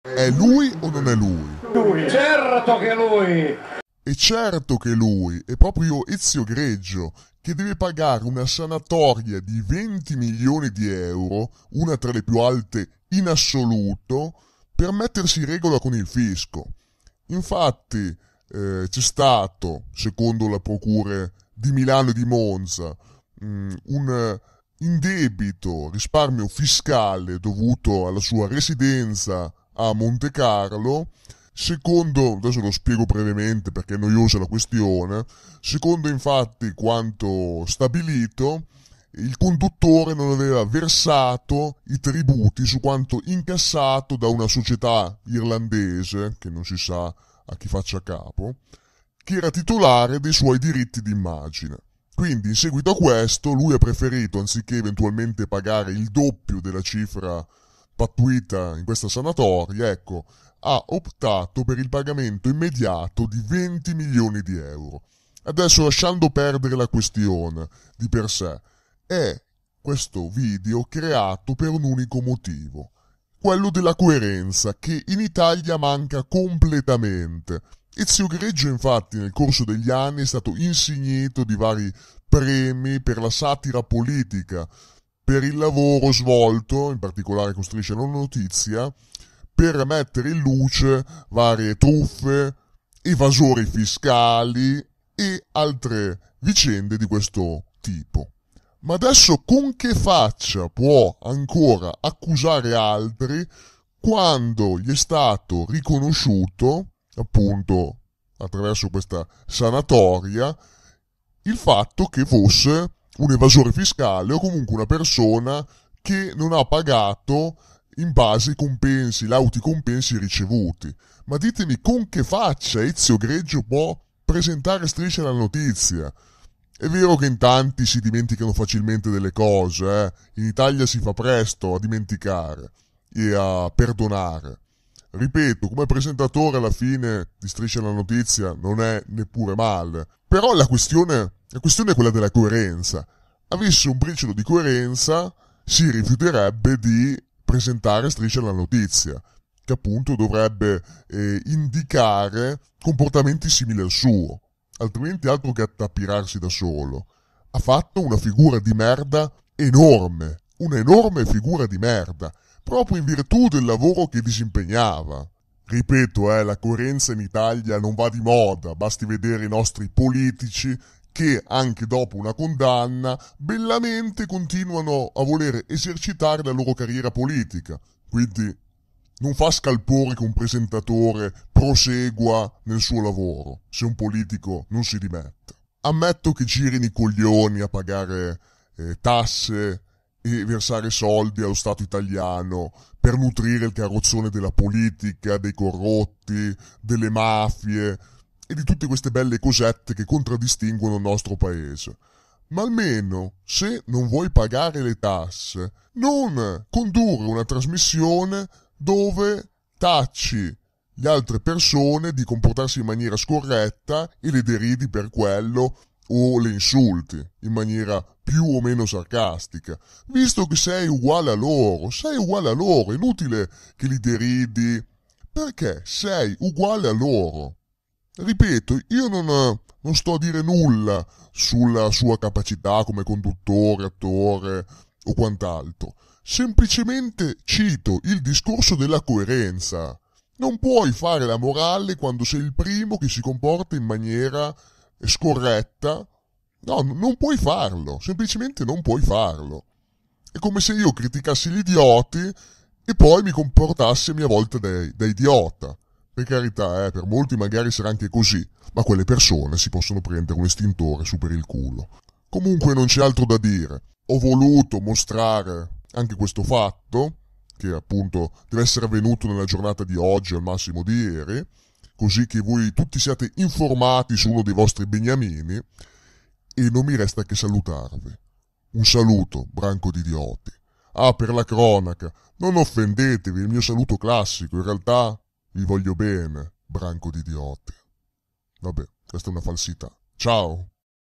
Ezio Greggio, 20 milioni al Fisco! Evasione fiscale e tasse non pagate! Un tapiro per la coerenza.
Il famoso presentatore di Striscia la Notizia, Ezio Greggio, deve pagare una maxi sanatoria da 20 milioni di euro, per tasse non pagate! Ecco cosa ne penso.